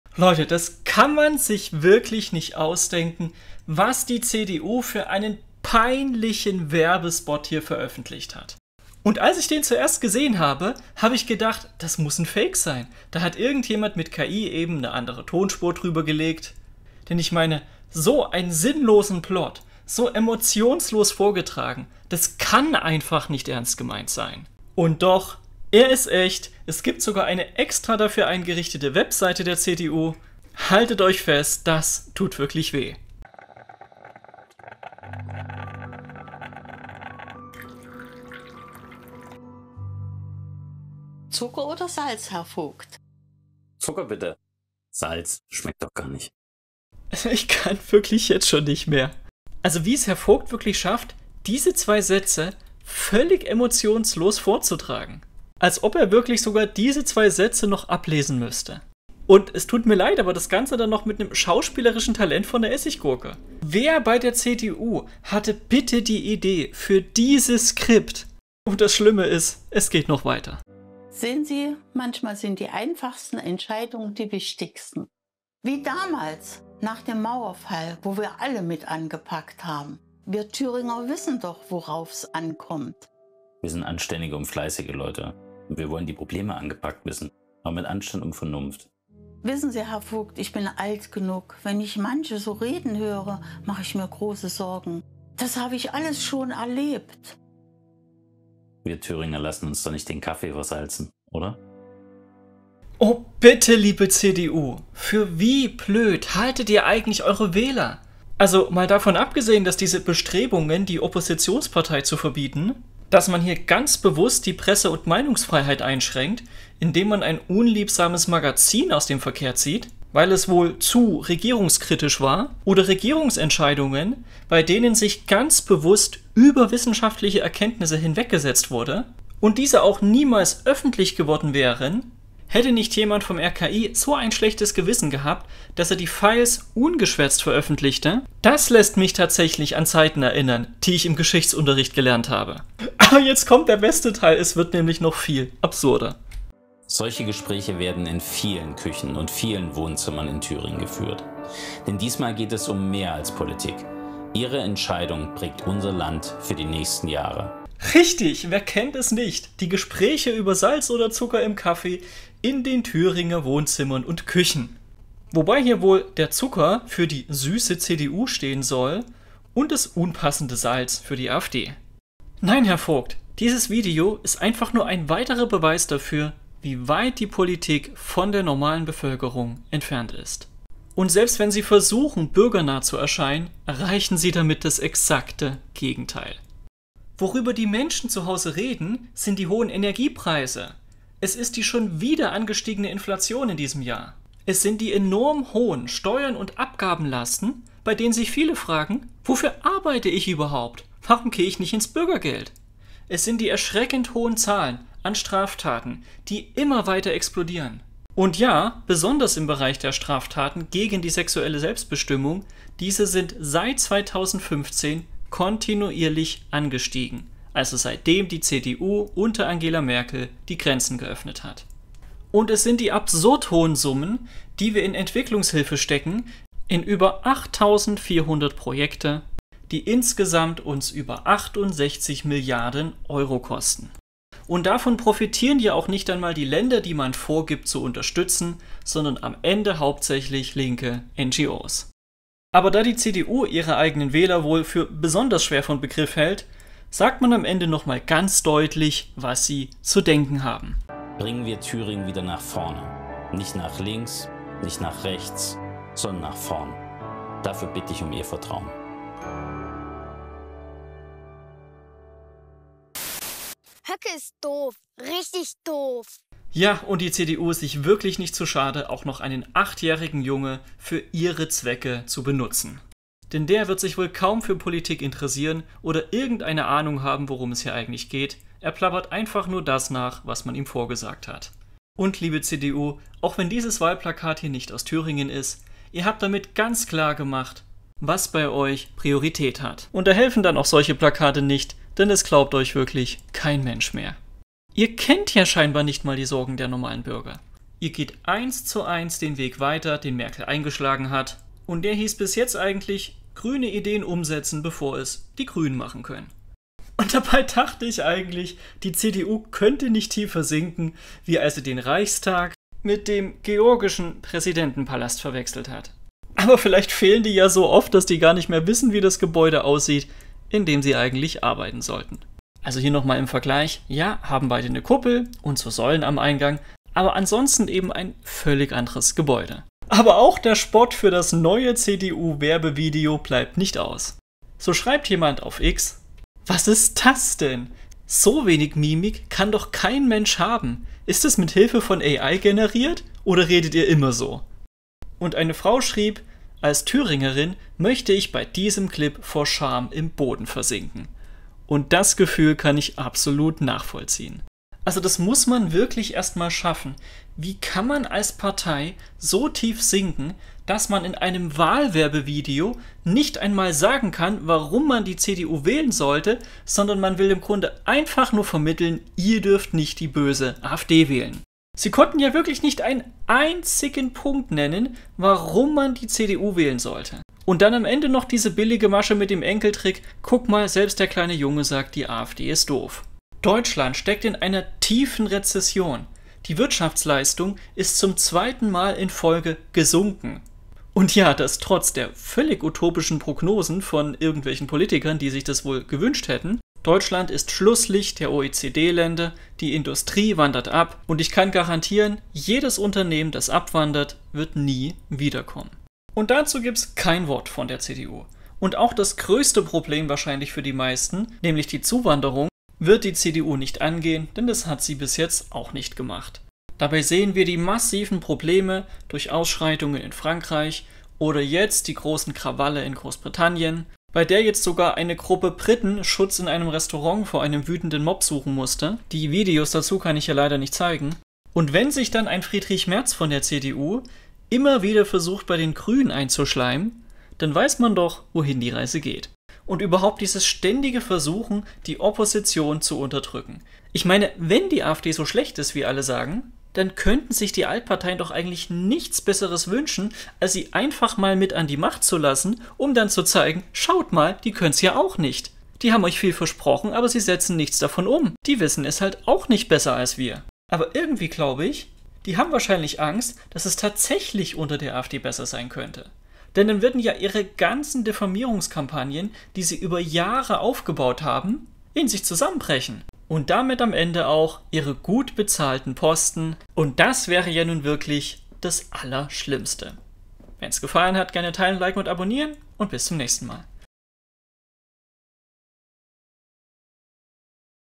Wähler: "Man möchte vor Scham im Boden versinken!" 🤣
CDU macht peinlichen Wahlwerbespot.

▬ Spenden ▬▬▬▬▬▬▬▬▬▬▬▬
BTC: bc1qvs8sfch8n2j0xstacemrq7j7vcljq6vcxz55d8
ETH: 0xFD08E9e8Dc5464957e90244F2EABf69Aea355774
LTC: ltc1q8zf3ks7w6ppytk8rulq0qzhre4yxjww8fq7ep7